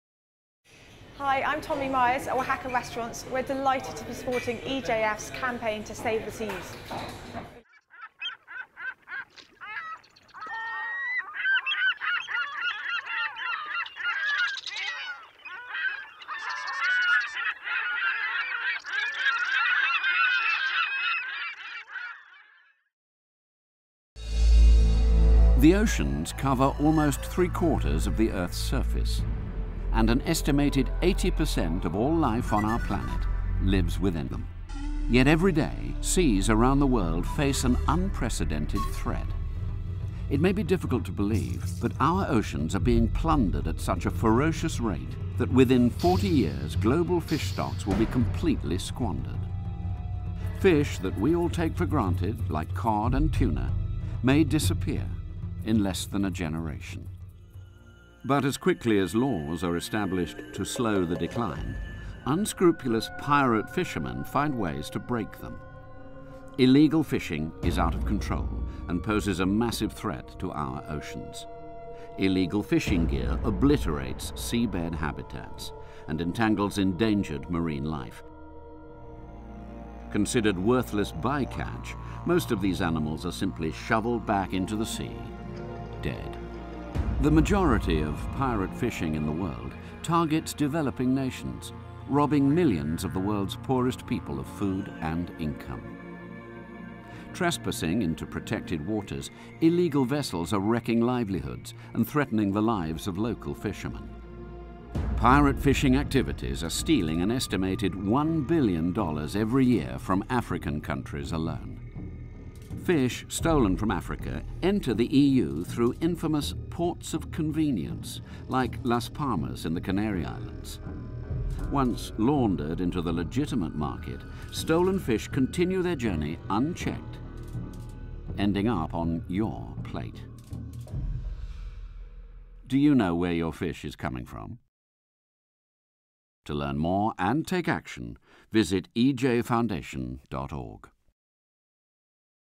Tommi from Wahaca's Save the Sea message
Wahaca's Tommi Miers shows her support for EJF's Save the Sea campaign at the restaurant in Covent Garden